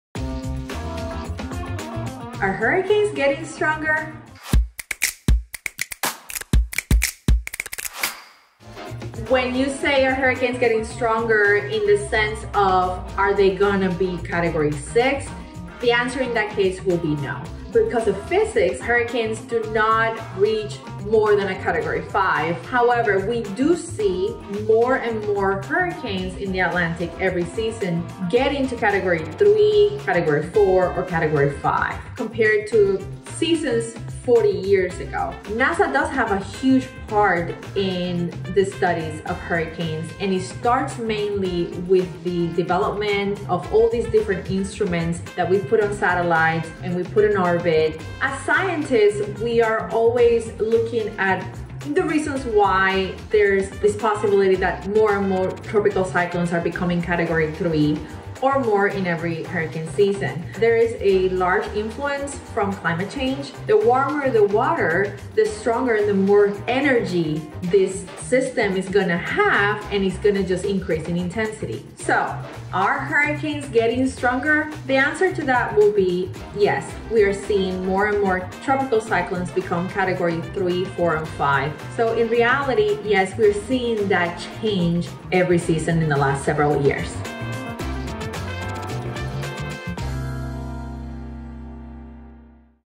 Are Hurricanes Getting Stronger We Asked a NASA Scientist
Are hurricanes getting stronger? Although we’ll never see a Category 6 hurricane, data does show that more hurricanes are becoming more severe. Hurricane and climate expert Mara Cordero-Fuentes of NASA's Goddard Space Flight Center tells us more about the connection between climate change and tropical cyclones. Producers: Scott Bednar, Jessica Wilde Editor: Daniel Salazar Credit: NASA